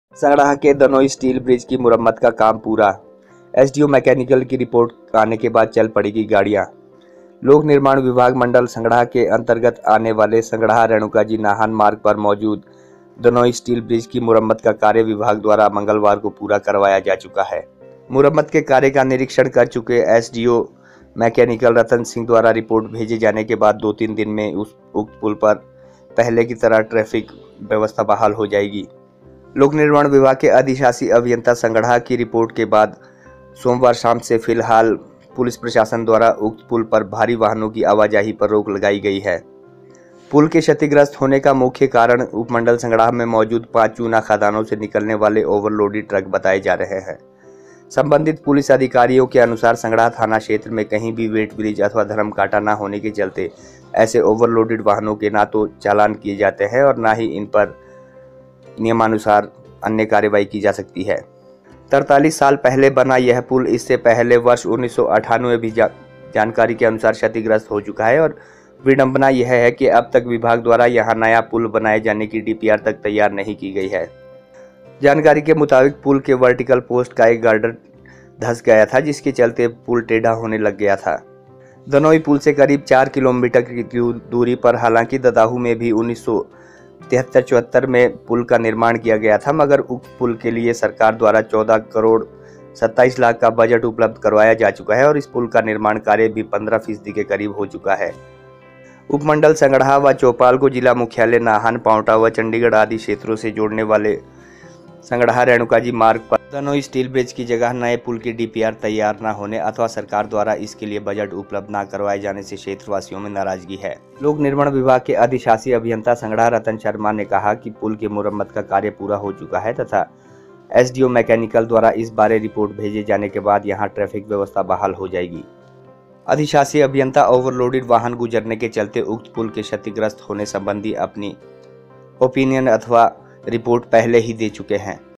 47 साल के दनोई Steel Bridge की repair तो हो गई
संगड़ाह-रेणुकाजी-नाहन मार्ग पर करीब 47 साल पुराने Steel Bridge की मुरम्मत तो हो गई, पर इसे Overloaded Limestone Truck से बचाना बड़ी जिम्मेदारी है। लोक निर्माण विभाग के अधिशासी अभियंता संगड़ाह रतन शर्मा ने बताया कि, मंगलवार को मुरम्मत कार्य पूरा हो चुका है और SDO मेकैनिकल की रिपोर्ट आना बाकी है।